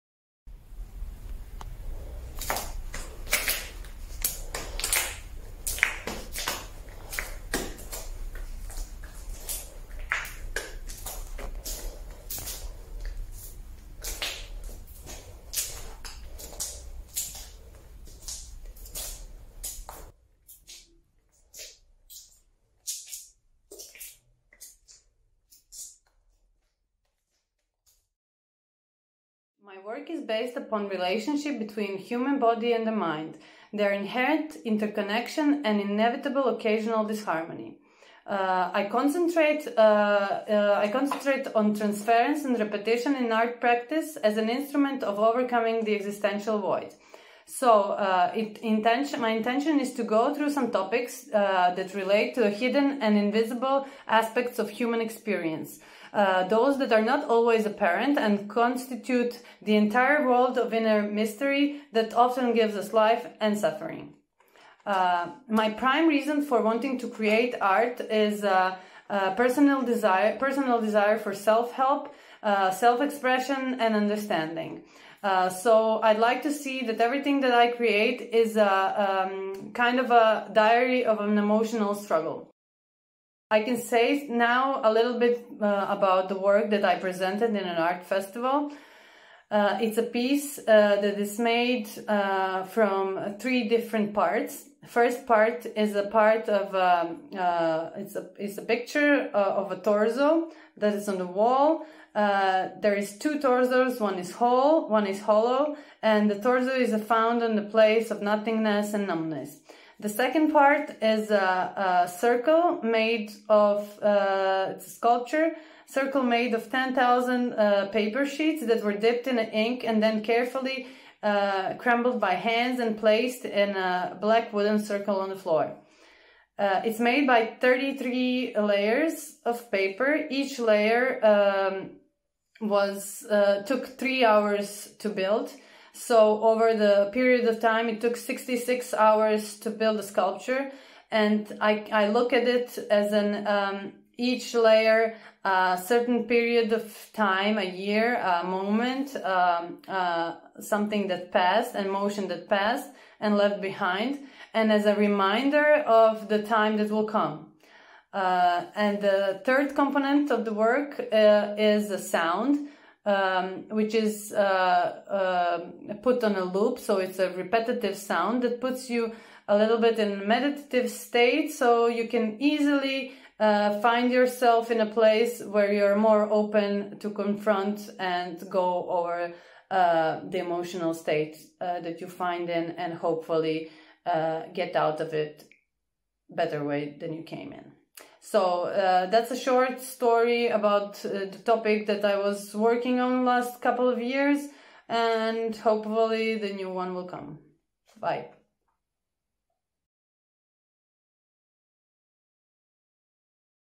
Through / Jovana Vujović
Transference and Repetition in an Art Practice as an Instrument of
Overcoming the Existential Void

My work is based upon the relationship between the human body and the mind, their inherent interconnection and inevitable occasional disharmony.
Every process and every creation cycle is a self-portrait in a sense.
The starting point of this personal process comes from a place of nothingness, of blank desperation and senseless lack of knowledge, followed by simple and straightforward physical actions, ones that create a relation of the body, materials and mind.By diving into the creative process, by pushing myself and stepping into the unknown, I glimpsed into states that allowed me to communicate with myself in a more direct and meaningful manner. Stepping out of the box, to see what the box really looks like. Gaining new perspective is as powerful a transformation tool as I can imagine.

This particular work is combined of 3 parts. First is a picture of a torso that is in a place of nothingness and numbness. One is whole the other is hollow.
Sculpture reflects the process of the mental state of the body.
It is made from 10 000 paper sheets dipped in ink and that crumpled by hands and pressed by feet placed on black circle made from wood.
It contains 33 layers, every layer is 3 hour of non stop repetitive work and trough that repetition you could find yourself lingering trough your mind.
Repetitive sound that is present puts you in some kind of meditative state where you could explore deeper parts of you.
Every paper, every layer felt like a little part of the soul,  a moment in life that is taken and put away, the time that is passed until now and the reminder of the time that will come.
Process is a piece in itself.